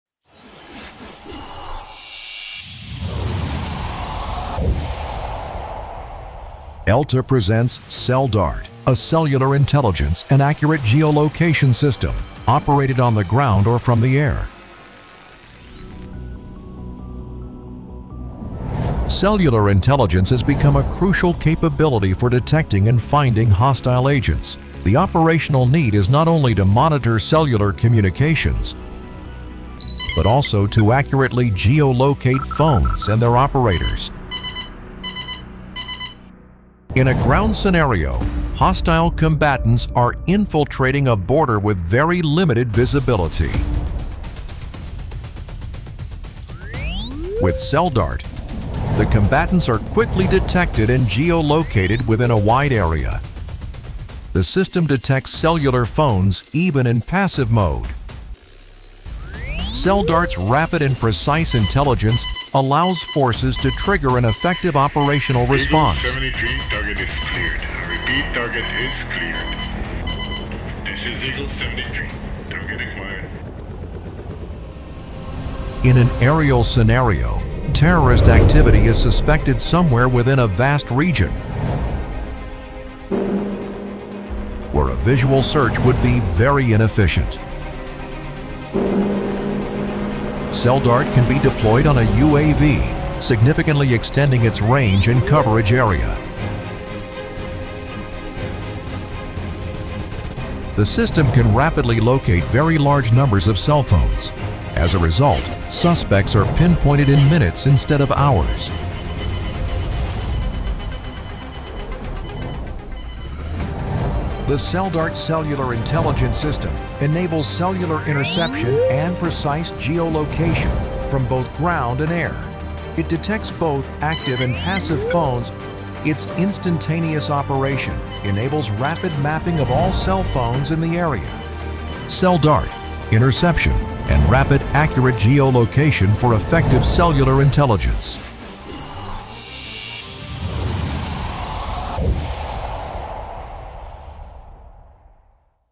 IAI-ELTA Systems Ltd. - - Celldart - active and passive cellular interception & DF
CellDart (ELK-7077) is an active and passive cellular interception and accurate geolocation system supporting 2G, 3G, 4G and 5G networks. It is a proven solution, based on unique, patented technology, and in operation with customers worldwide. The system is suitable for Intelligence, Surveillance and Reconnaissance (ISR) activities, border and maritime protection, HLS and law enforcement.